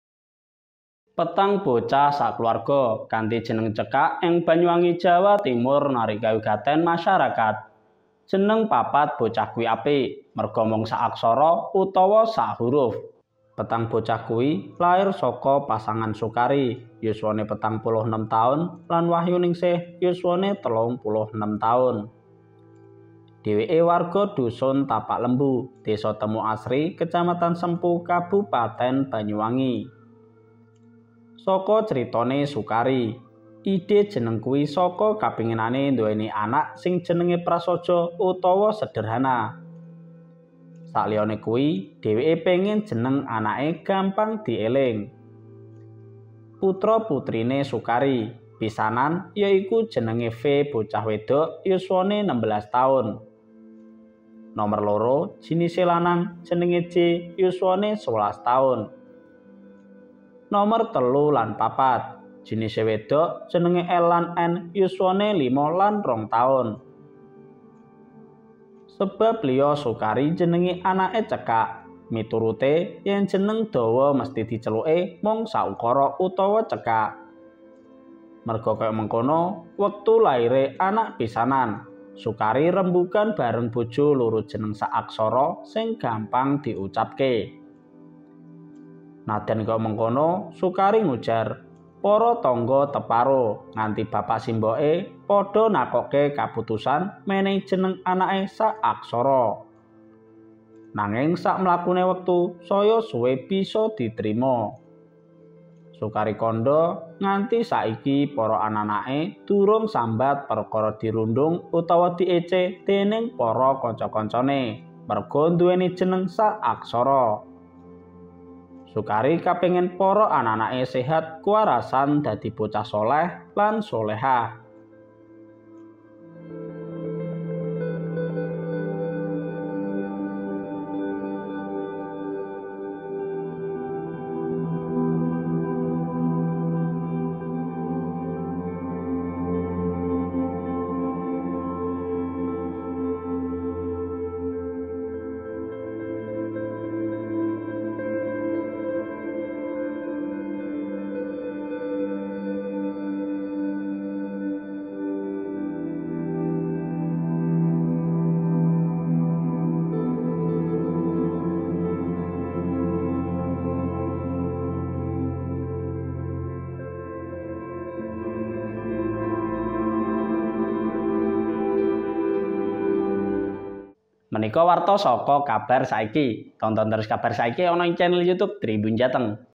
Empat Anak Bersaudara di Banyuwangi Punya Nama Satu Huruf
Petang bocah sak kulawarga kanthi jeneng cekak ing Banyuwangi Jawa Timur narik kawigaten masyarakat.

Jenenge papat bocah kuwi apik, merga mung sak aksara utawa sak huruf.

Petang bocah kuwi lahir saka pasangan Sukari (46) lan Wahyuningsih (36).

Dheweke warga Dusun Tapak Lembu, Desa Temuasri, Kecamatan Sempu, Kabupaten Banyuwangi.